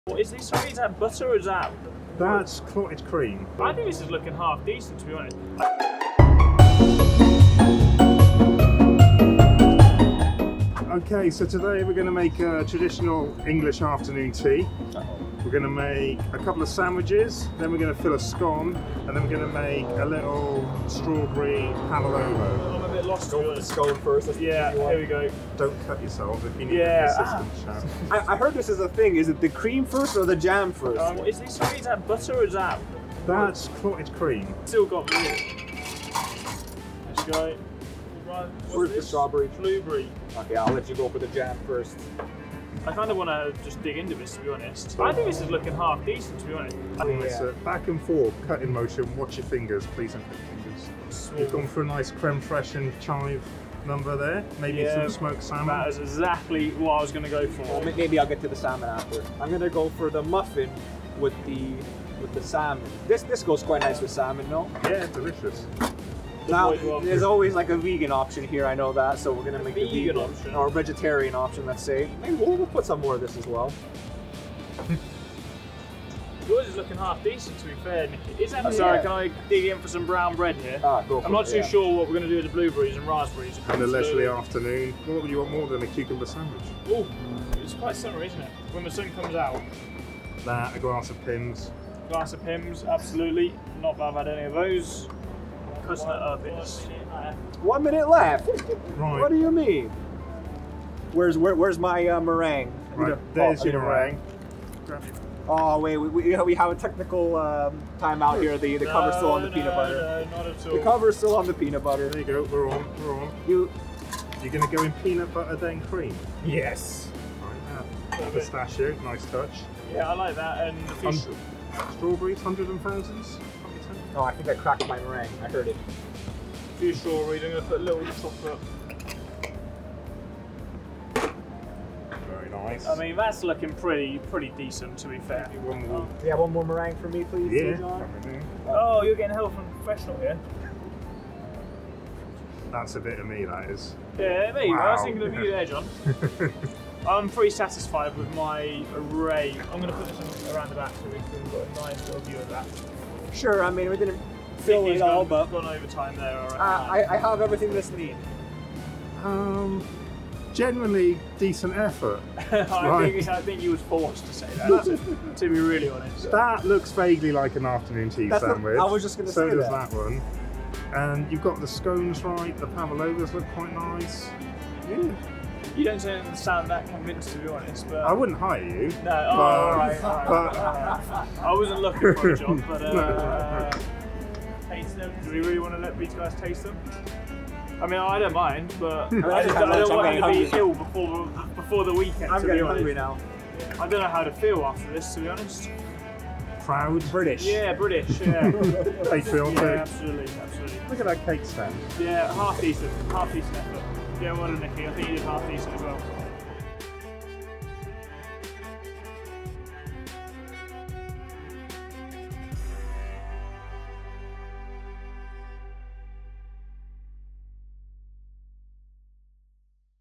Head 2 Head | Traditional British Afternoon Tea | Williams Racing
Whilst in Great Britain at the Silverstone Circuit, it’s only fitting that we put George Russell and Nicholas Latifi to the test in an afternoon tea making competition. Who will be the winner of our first Head 2 Head competition?